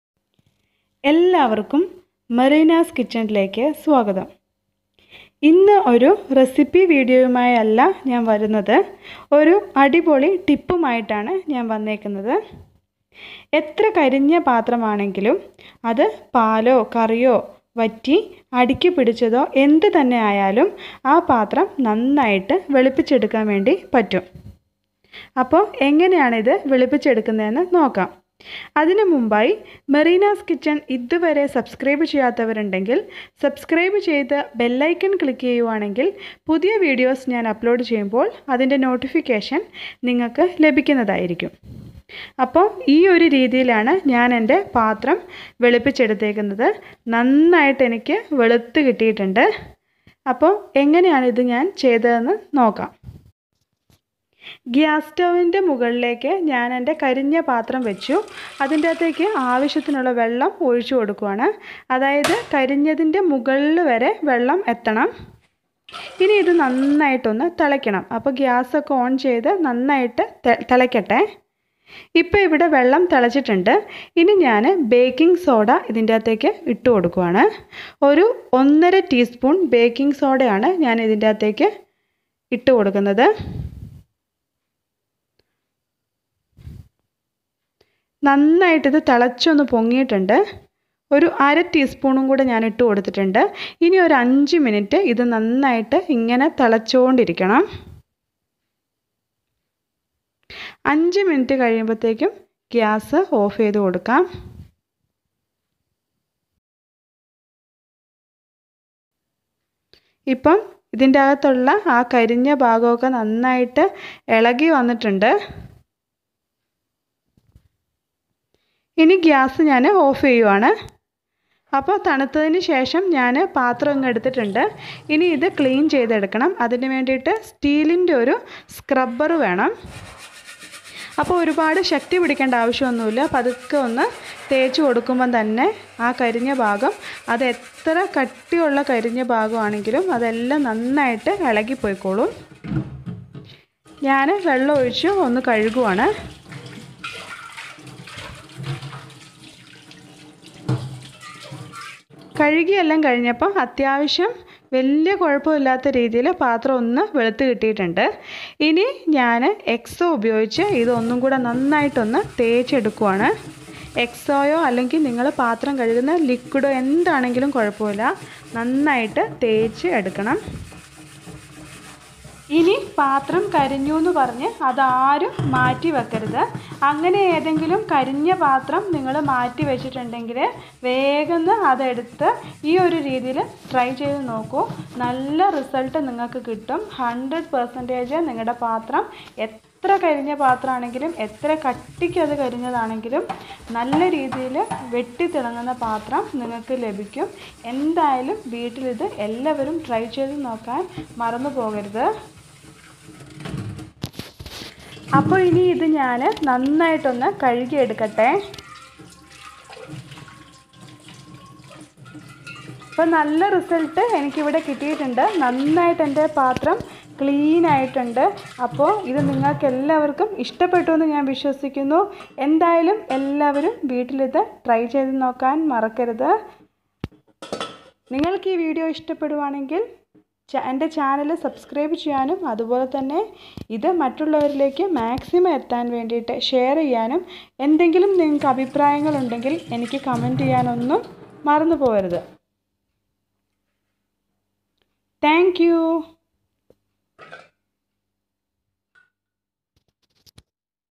സോഡാ പൊടി ഉപയോഗിച്ച് ഏത് കരിഞ്ഞ പാ८തവും വൃത്തിയാക്കാം || mereena's kitchen
ഏത് കരിഞ്ഞ പാ८തവും സോഡാ പൊടി കൊണ്ട് വൃത്തിയാക്കാം എന്നതാണ് ഇന്നത്തെ വീഡിയോ.